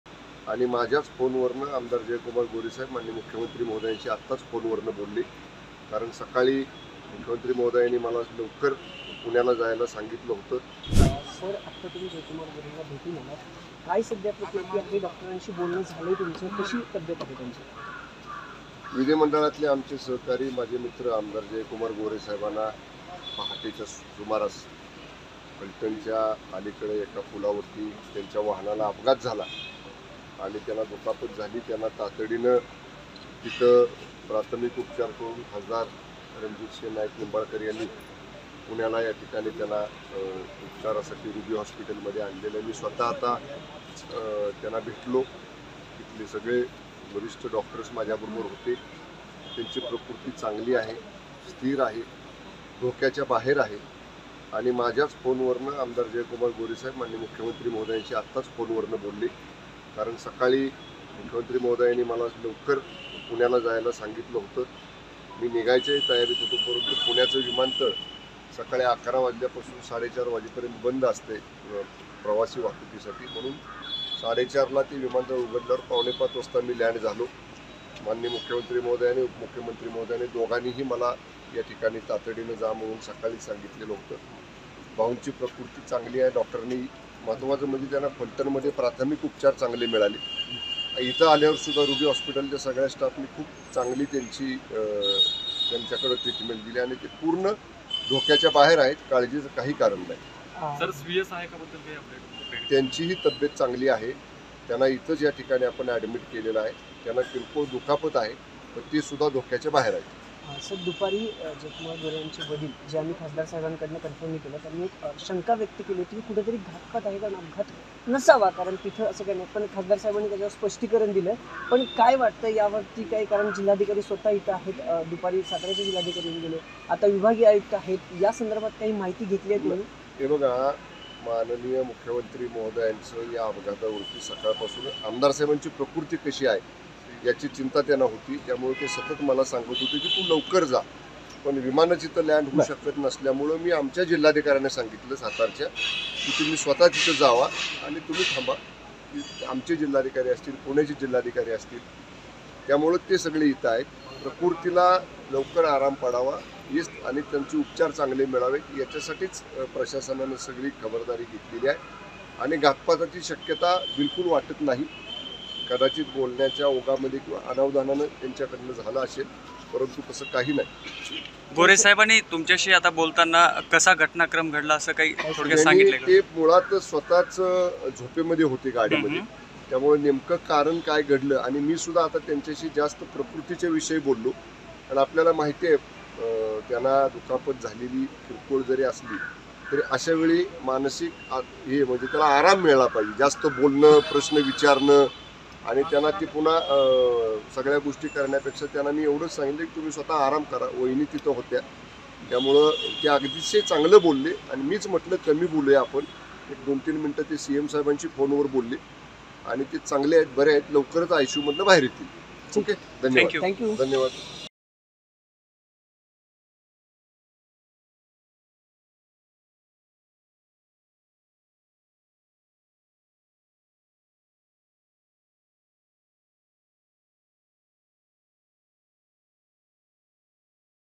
Shambhuraj Desai: मुख्यमंत्री शिंदेंनी केली आमदार गोरेंच्या तब्येतीची विचारपूस |LetsUpp Marathi
उत्पादन शुल्क मंत्री शंभूराज देसाई हे आमदार जयकुमार गोरे यांच्या तब्येतीची विचारपूस करण्यासाठी रुग्णालयात गेले होते. यावेळी मुख्यमंत्री एकनाथ शिंदेंनी मंत्री शंभूराज देसाई यांच्या फोनवरून आमदार गोरेंच्या तब्येतीची विचारपूस केली.